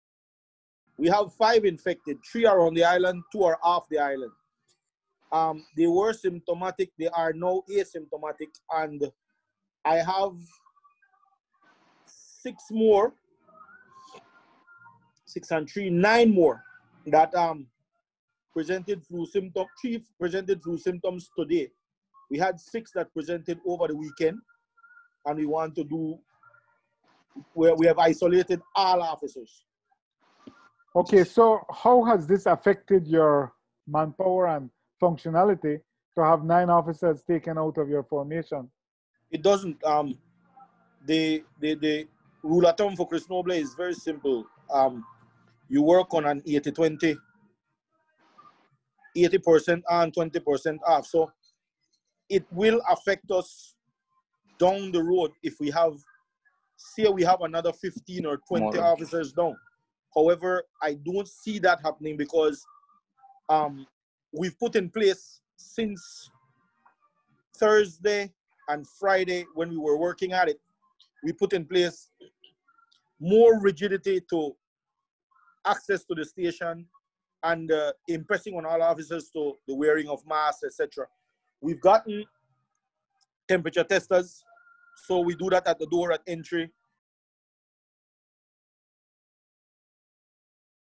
COVID-19 Hits San Pedro Cops!
In San Pedro, fourteen officers attached to the San Pedro Police Station are in isolation.  Of those fourteen, five have tested positive for COVID-19; three of whom are on the island, while the other two are on the mainland.  The remaining officers are exhibiting flu-like symptoms.  Today, Officer in Command in San Pedro, Superintendent Chris Noble says that it is unclear from where or which cluster the officers contracted the virus.  Noble says that the department has been providing as much support to the officers as is possible.